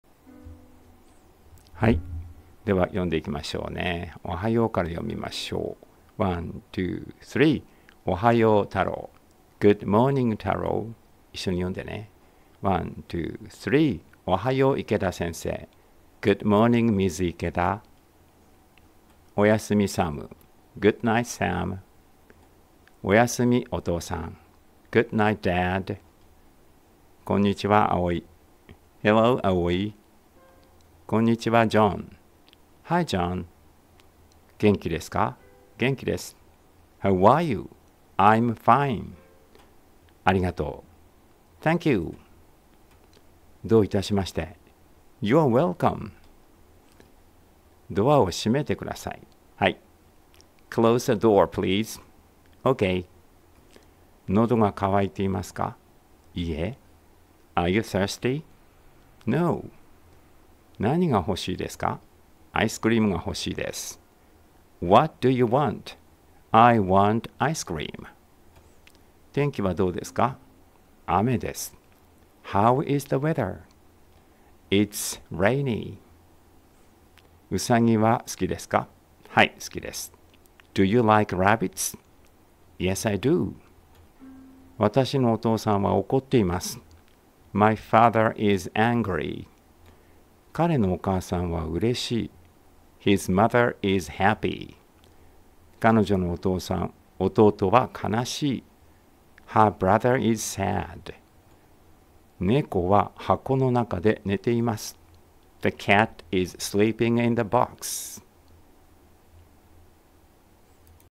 GTEC Junior1 れんしゅう おんどく
1　同じものを繰り返し音読しない
2　読めないところは、読まなくてもよい
3　発音に注意を払わない
4　声の大きさ、速さは自由
5　ほかの人の迷惑になりそうなときは、声を出さず、頭の中で音読する
6　音読の内容は何でもよい
7　音読をしたくないときはやらない
8　音読の順番に決まりはない

おはよう、たろう。
Good morning, Taro.

おはよう、いけだせんせい。
Good morning, Ms. Ikeda.

おやすみ、さむ。
Good night, Sam.

おやすみ、おとうさん。
Good night, Dad.

こんにちは、あおい。
Hello, Aoi.

こんにちは、じょん。
Hi, John.

げんきですか？ げんきです。
How are you? I'm fine.

ありがとう。

どういたしまして。

どあをしめてください。 はい。
Close the door, please. OK.

のどがかわいていますか？ いいえ。
Are you thirsty? No.

なにがほしいですか？ あいすくりーむがほしいです。
What do you want? I want ice cream.

てんきはどうですか？ あめです。
How is the weather? It's rainy.

うさぎがすきですか？ はい、すきです。
Do you like rabbits? Yes, I do.

わたしのおとうさんはおこっています。
My father is angry.

かれのおかあさんはうれしい。
His mother is happy.

かのじょのおとうとはかなしい。
Her brother is sad.

ねこははこのなかでねています。
The cat is sleeping in the box.